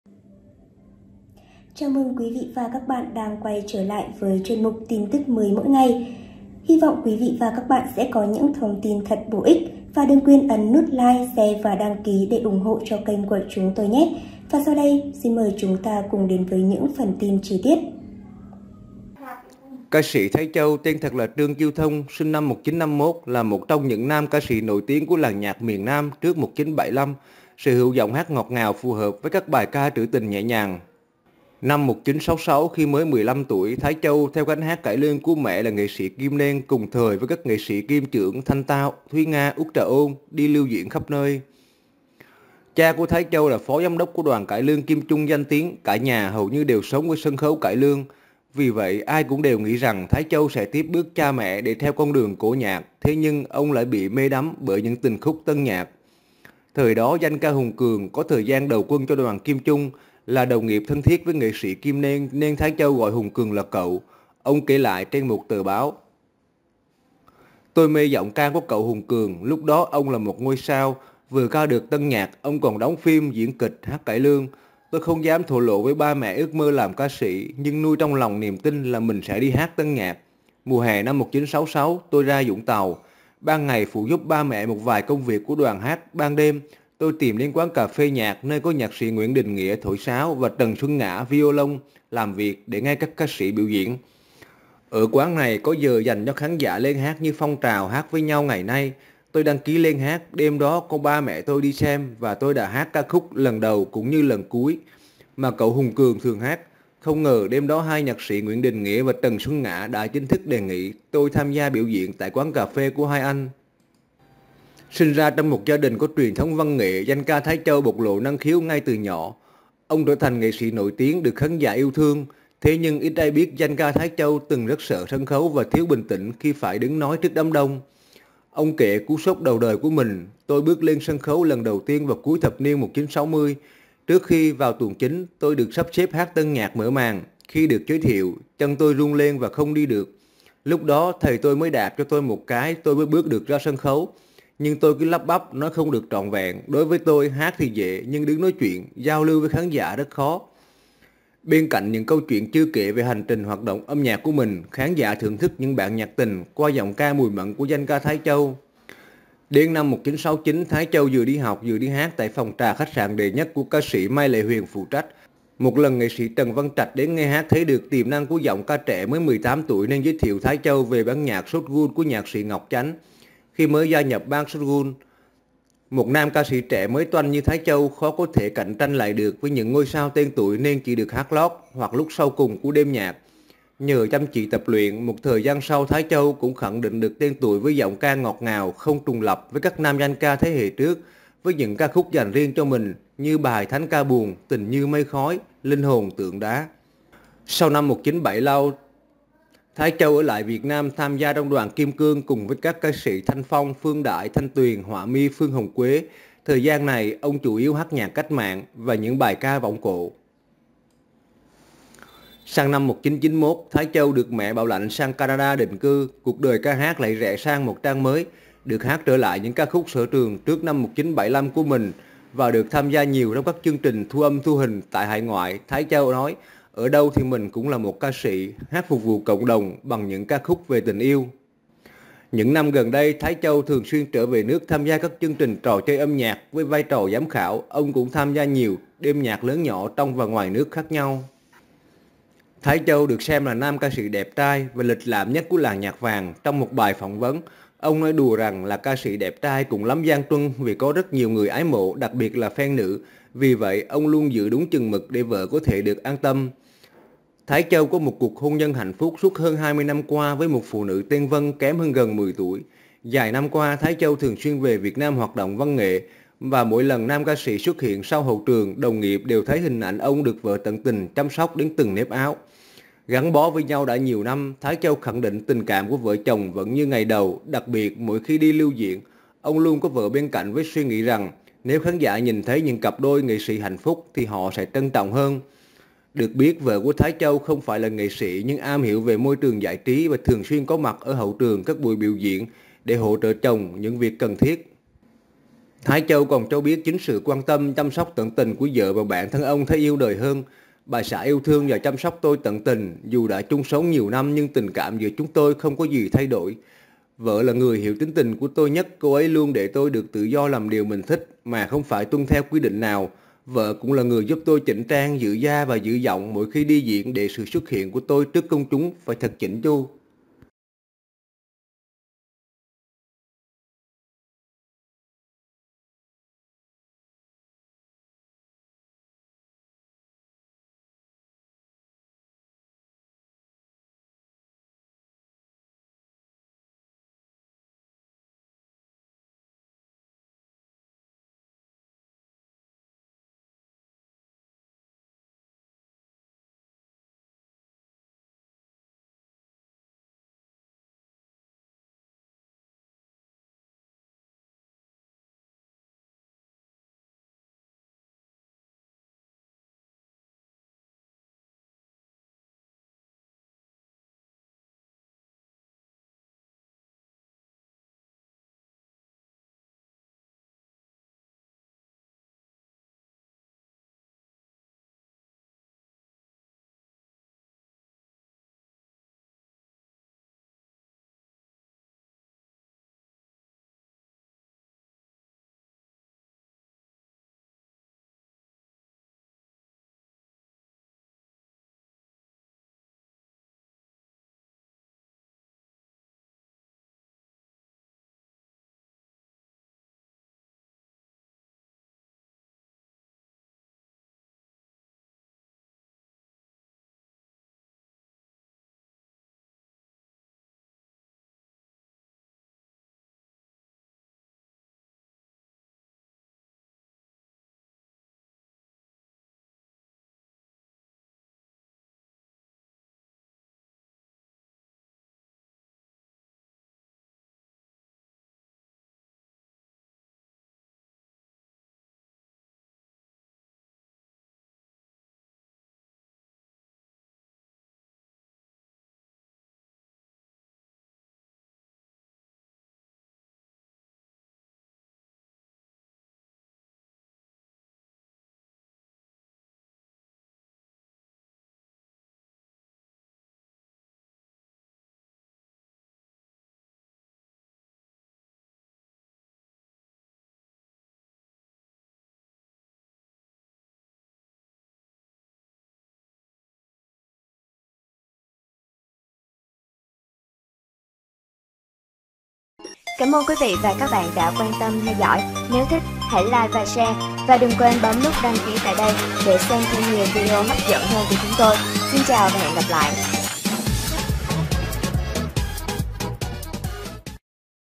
🔴DANH CA THÁI CHÂU ĐA TÀI CUỘC ĐỜI KHỔ CỰC Ở TUỔI XÉ CHIỀU ÍT AI BIẾT
🔴DANH CA THÁI CHÂU ĐA TÀI  CUỘC ĐỜI KHỔ CỰC Ở TUỔI XÉ CHIỀU  ÍT AI BIẾT TỚI